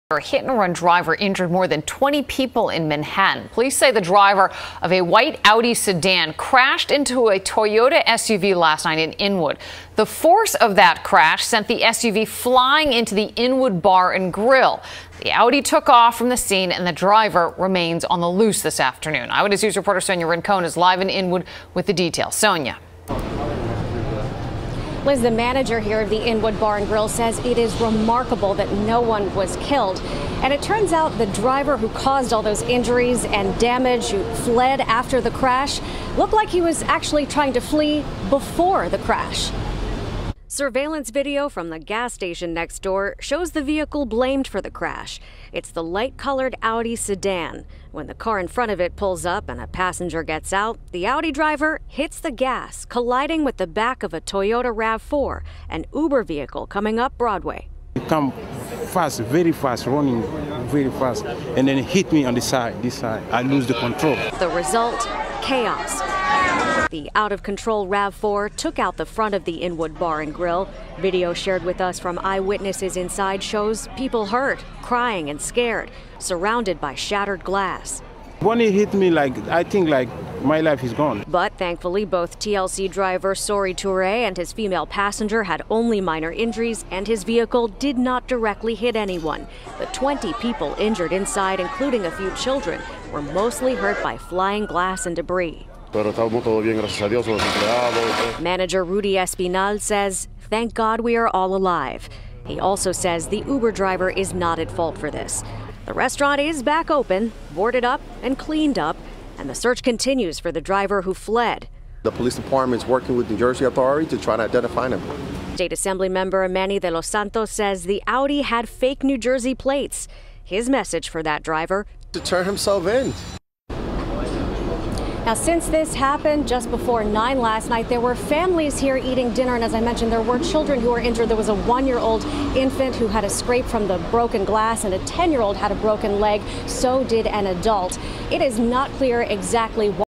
Car Crashes Into New York Restaurant, Injuring 22 People
Car Crashes Into New York Restaurant, Injuring 22 People and audi escapes seen still looking for suspects !!!